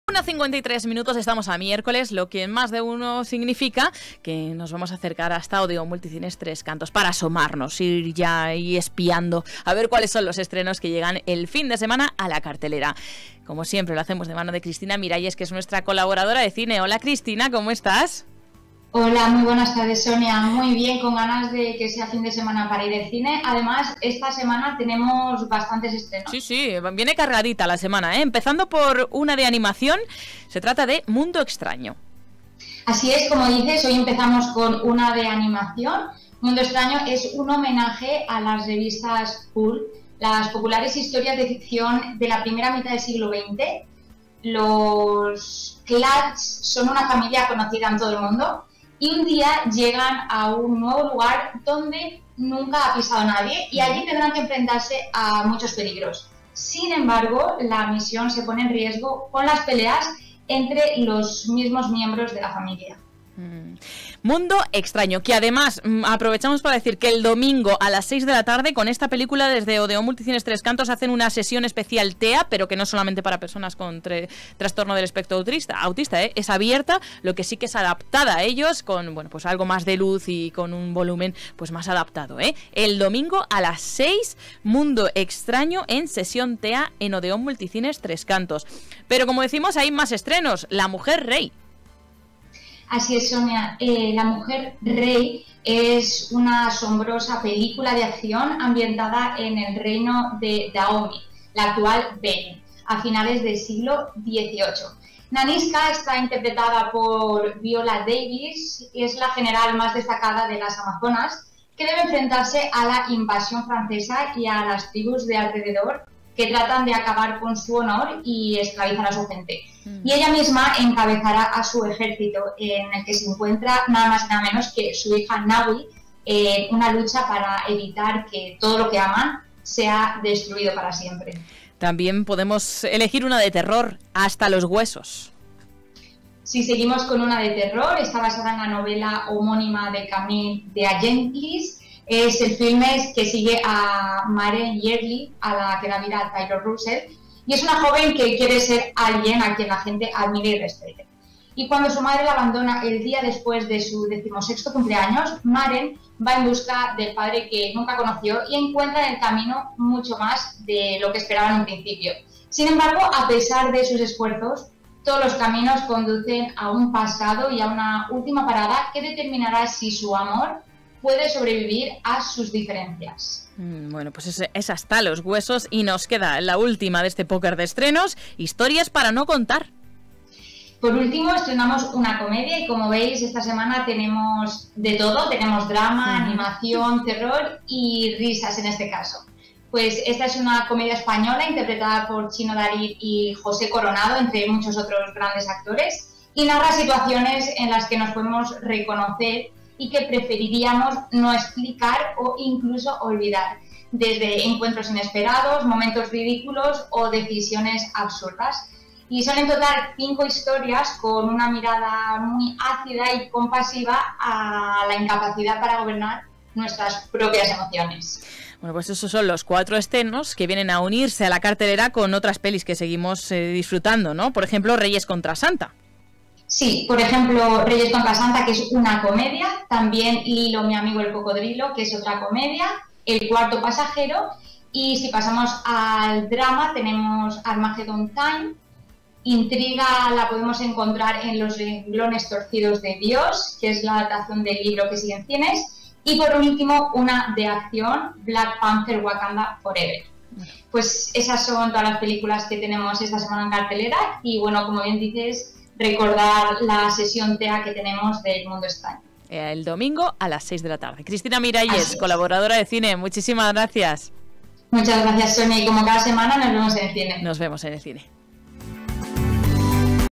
Estrenos de Cine Odeón Multicines Tres Cantos
repasaremos los estrenos de cine que llegan el viernes a Odeón Multicines Tres Cantos.